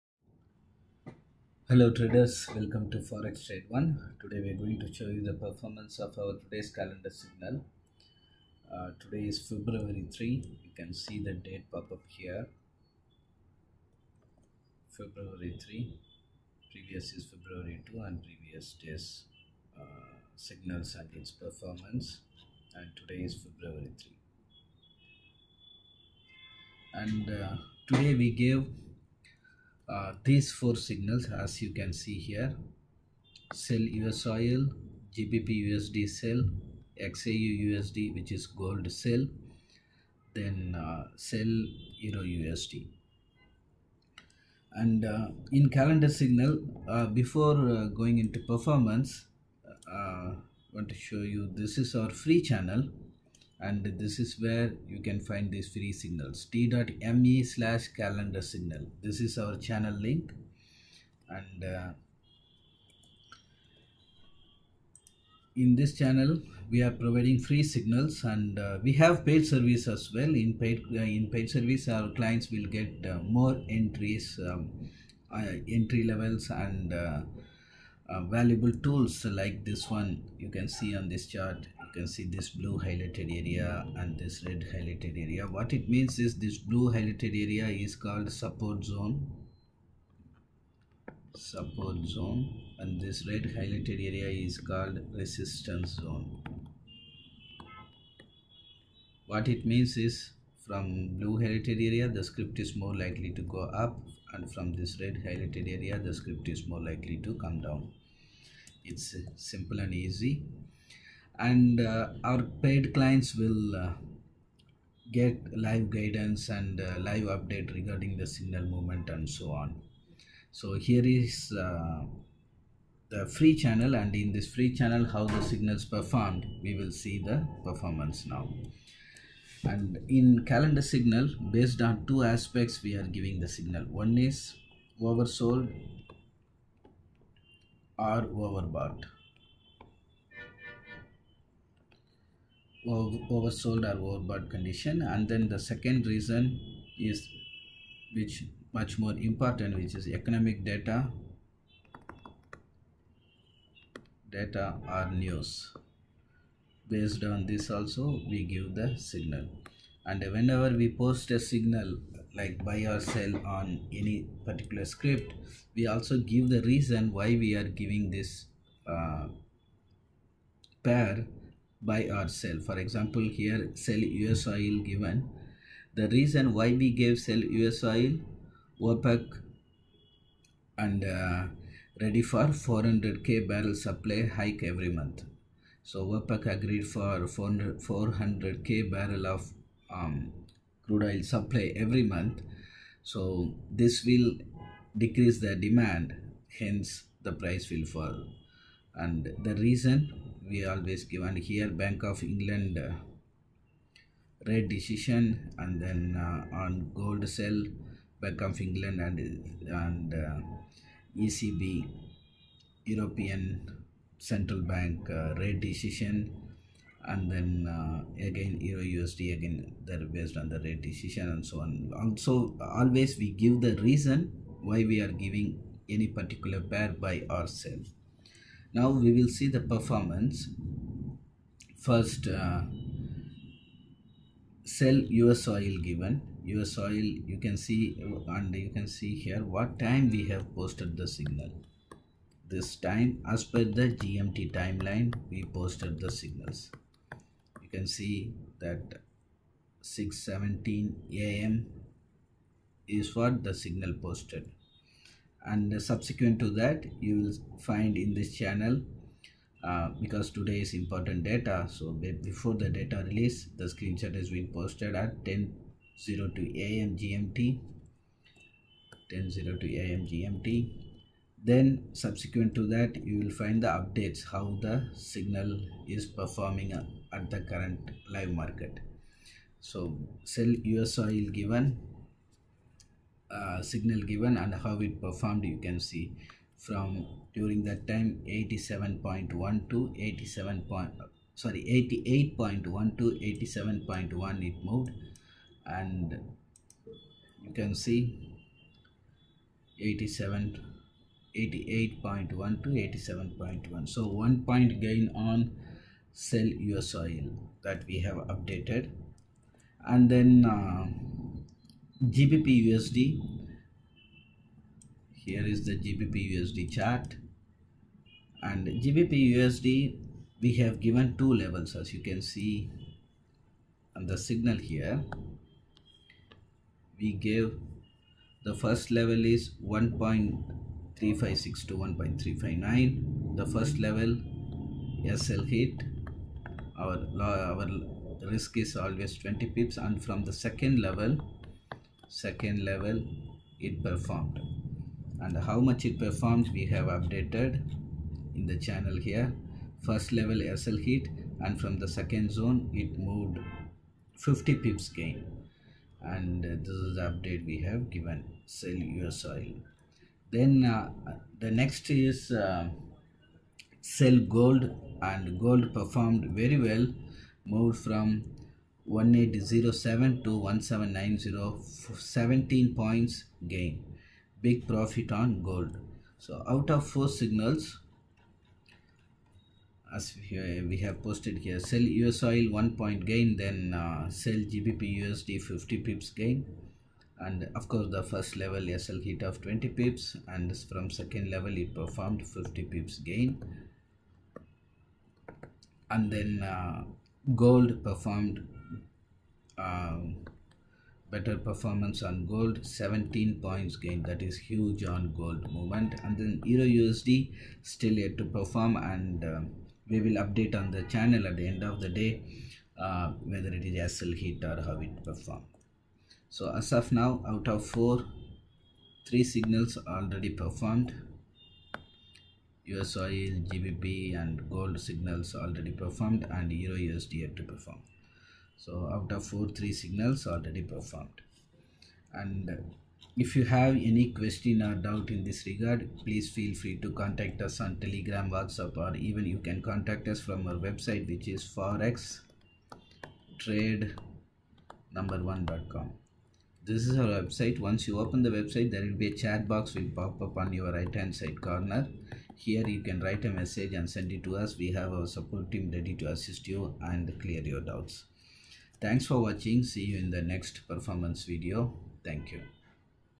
3.2.2022 CALENDAR SIGNAL PERFORMANCE VIDEO
» SELL USOIL » 1.0 Point Gain
» SELL GBPUSD » 1st Level SL hit 20pips loss and from 2nd Level 50pips gain
» SELL XAUUSD » 17 Points Gain